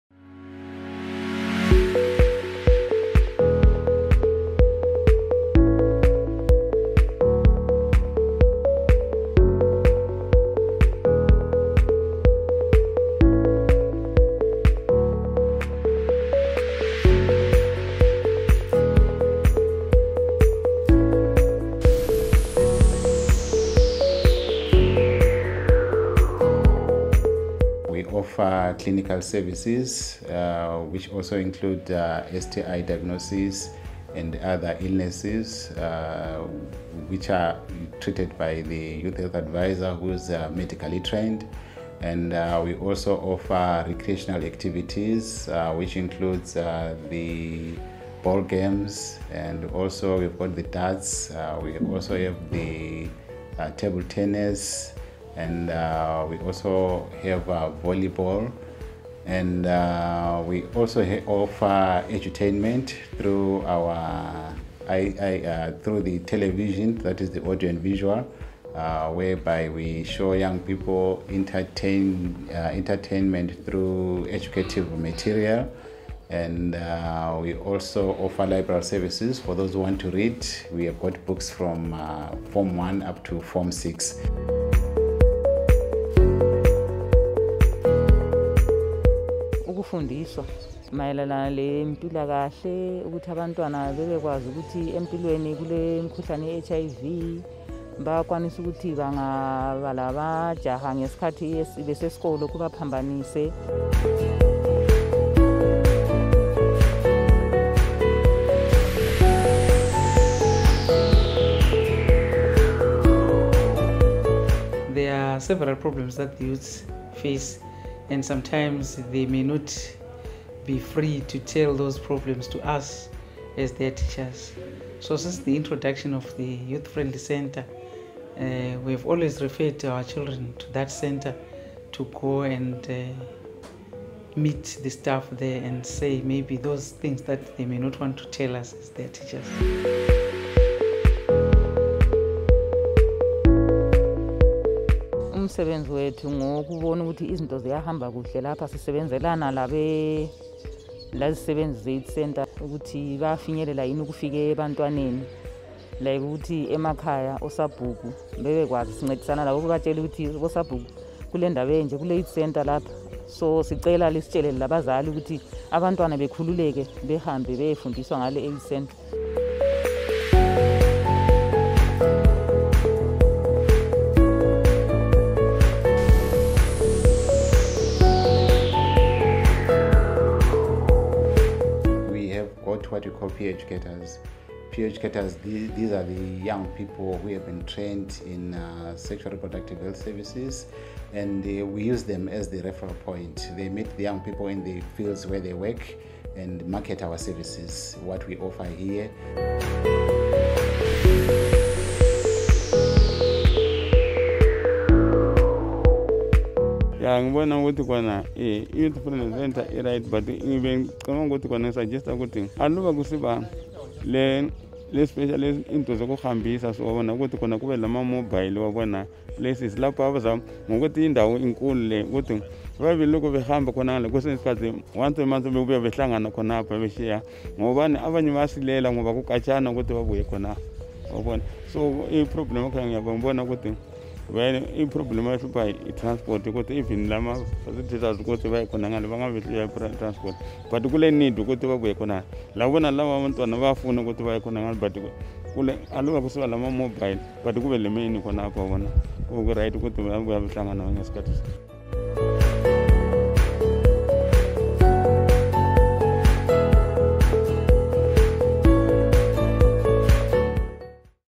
HD Zezani Youth Centre Documentation 2020
ZNFPC footprints are visible in the Matabeleland South province in Gwanda District where we work with the community at Zezani Youth-friendly Centre to help In and Out-of-school youths with life skills education, library facilities, computer literacy training, IEC materials and behaviour change communication among other youth-related initiatives.